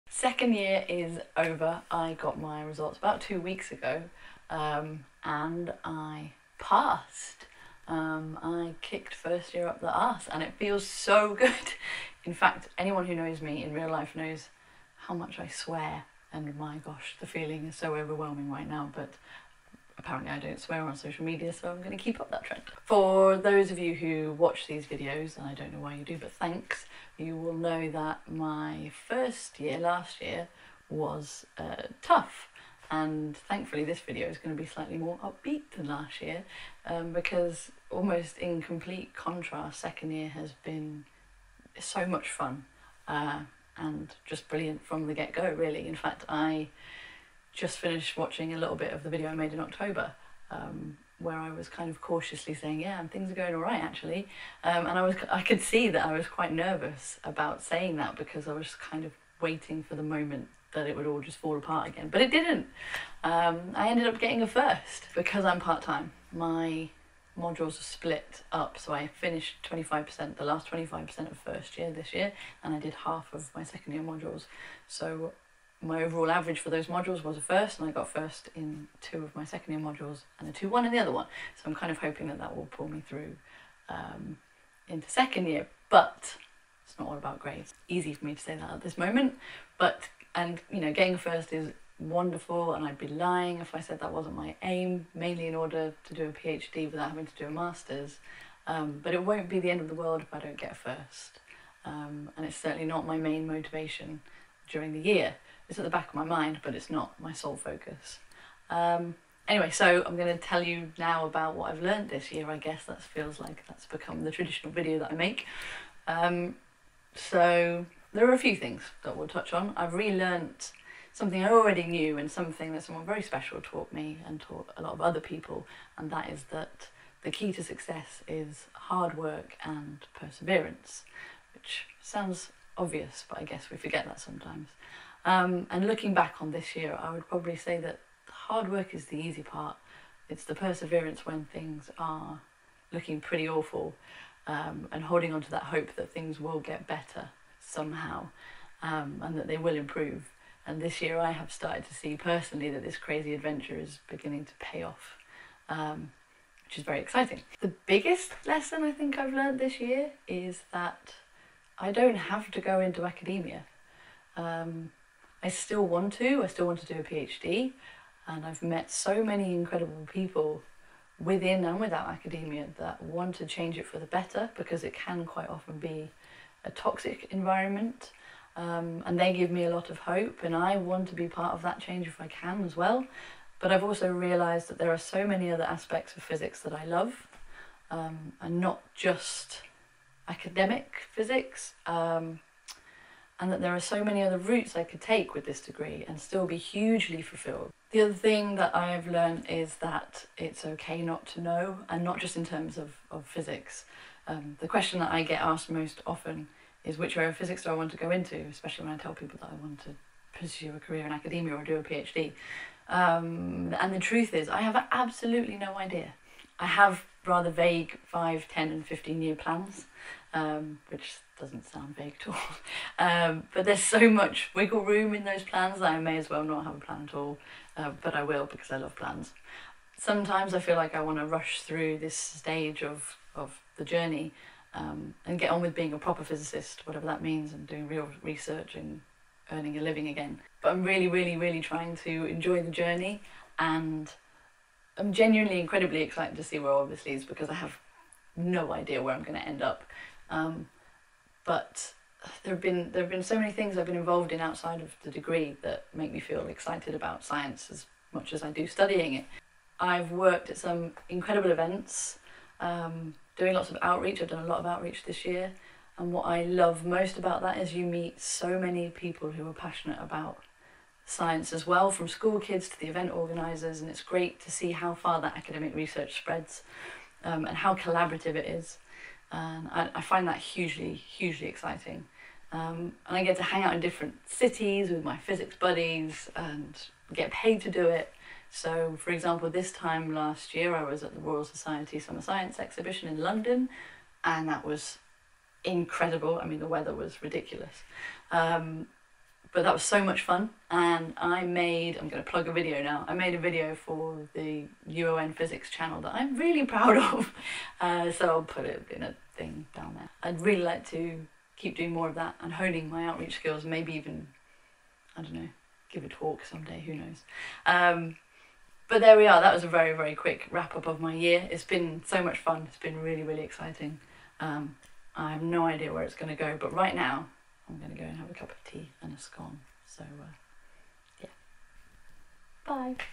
Results Day! And Lessons from Second Year
Second year is over! Only two years left. (And then another three...) The journey so far has been ace. Let's hope it stays that way!

Super awesome video I made about the Royal Society Summer Science Exhibition last year for the School of Physics and Astronomy at Nottingham